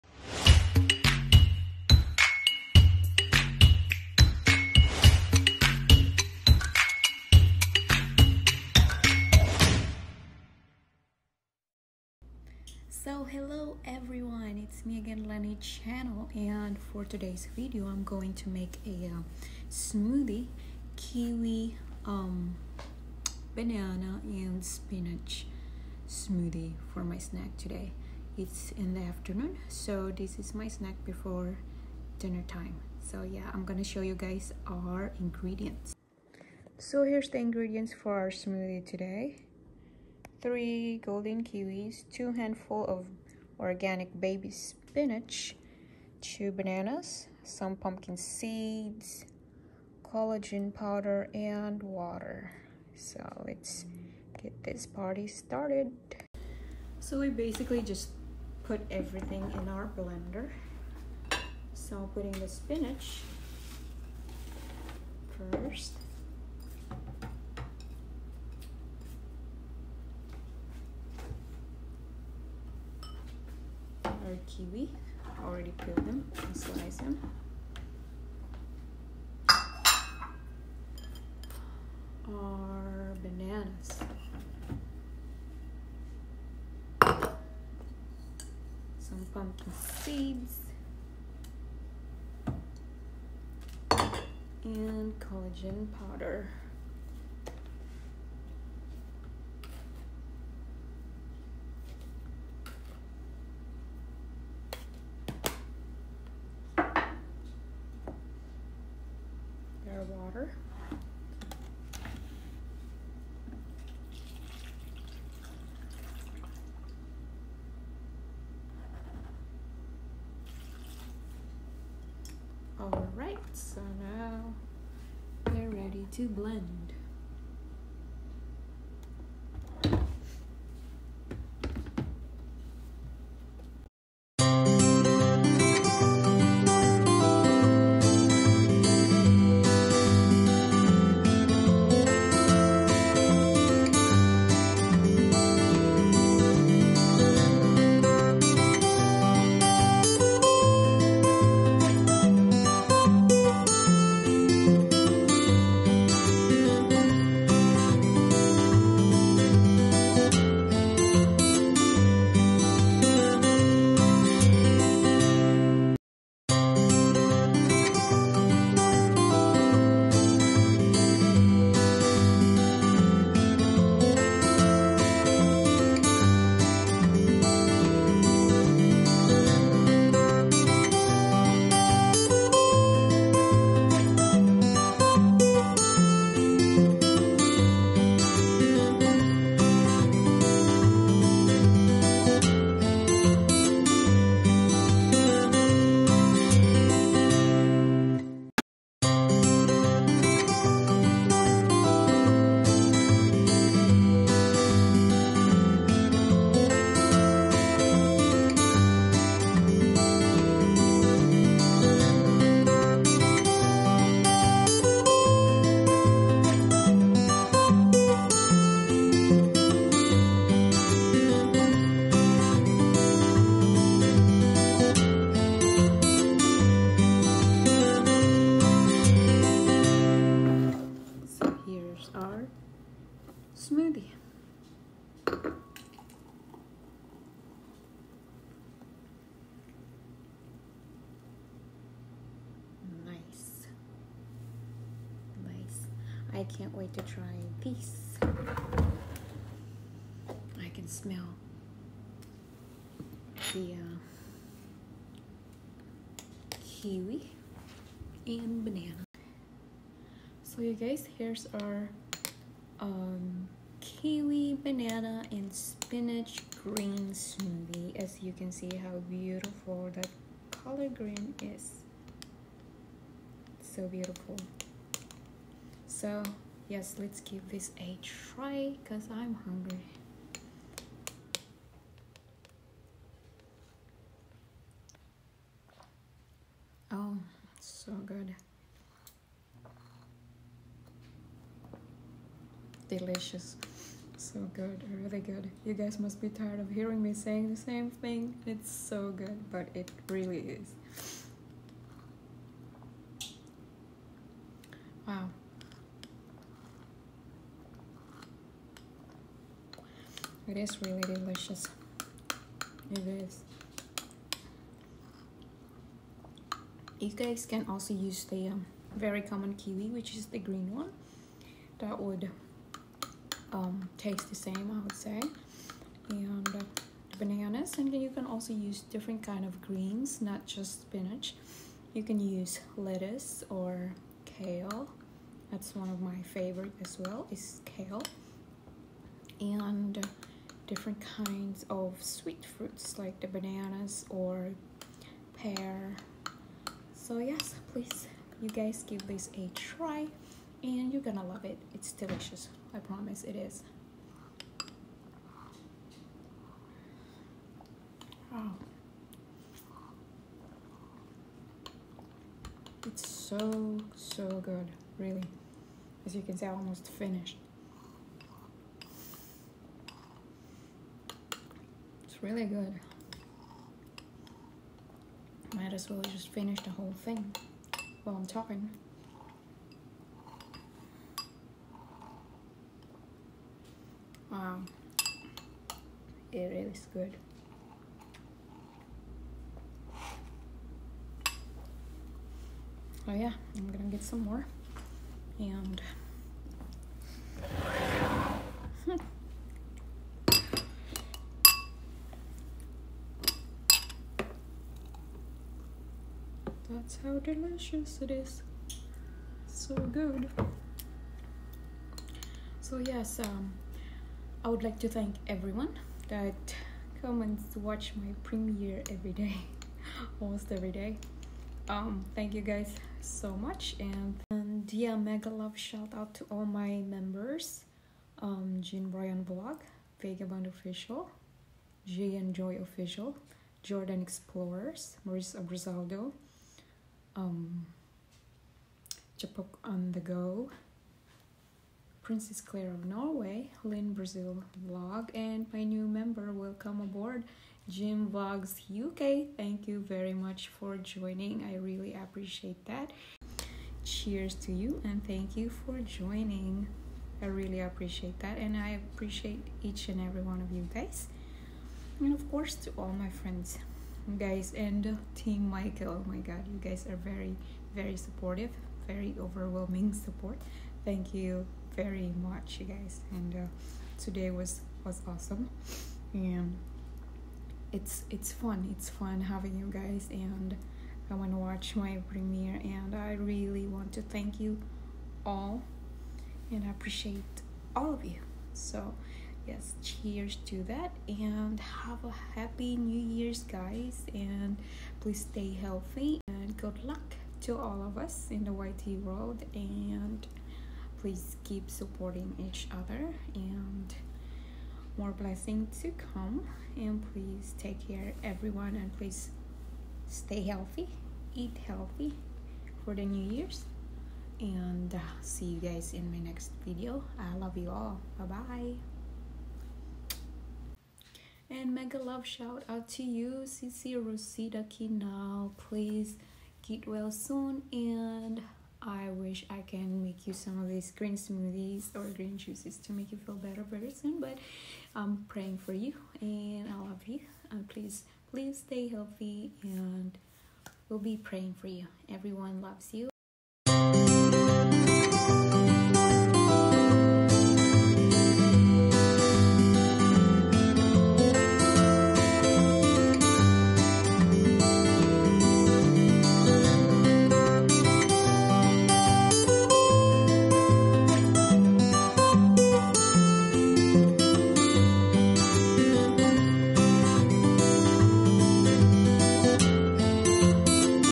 Kiwi Banana and Spinach green smoothie🥝 🍌🍌🥬
Smoothie Ingredients;

2 Bananas
3 kiwis
2 handfuls baby spinach
Pumpkin seeds
Collagen powder
Water

Blend everything and enjoy 😊